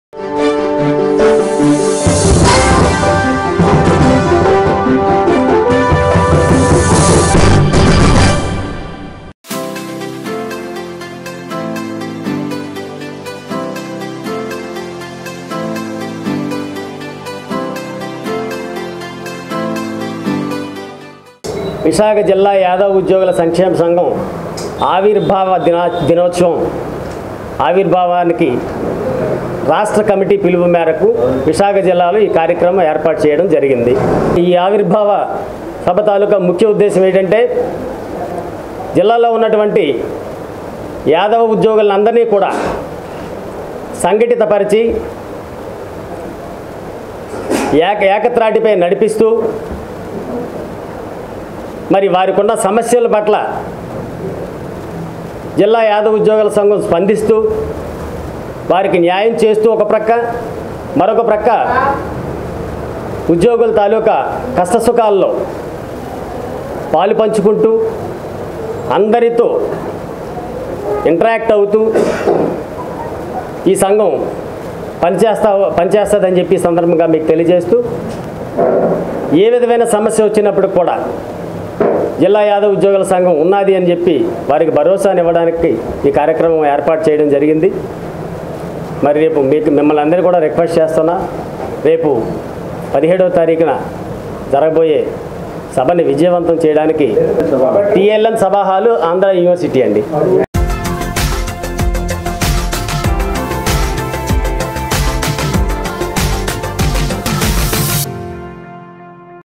smartcity: జూన్‌ 17న విశాఖ జిల్లా యాదవ ఉద్యోగుల సంక్షేమ సంఘం ఆవిర్భావ దినోత్సవ మహాసభ...
smartcity webnews: జూన్‌ 17న విశాఖ జిల్లా యాదవ ఉద్యోగుల సంక్షేమ సంఘం ఆవిర్భావ దినోత్సవ మహాసభ...